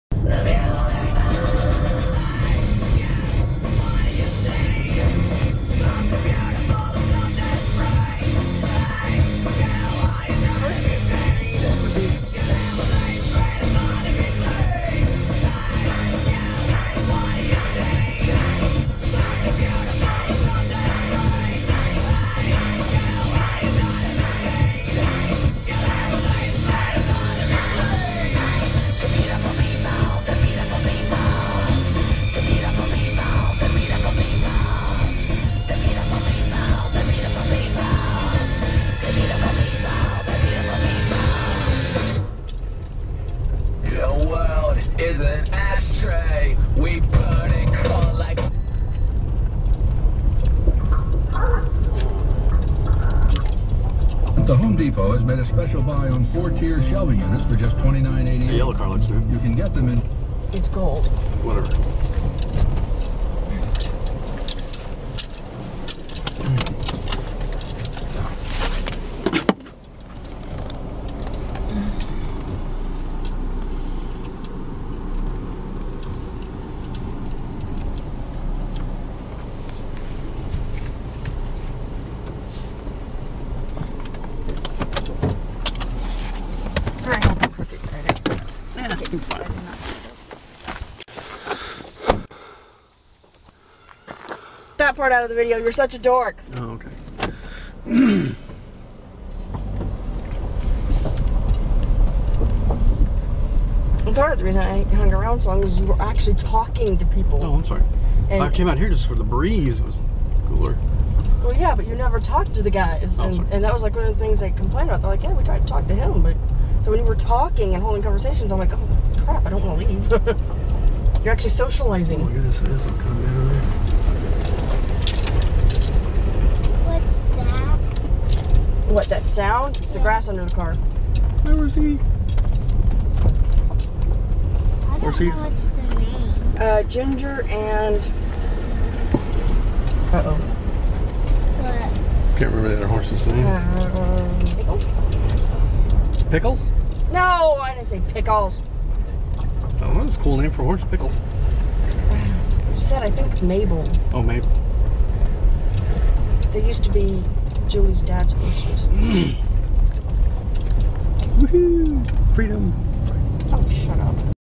Picking up Elle
This is a video entering and leaving Julie's driveway. God that sounds bad. The conversation at the end in the only reason I'm putting this video up.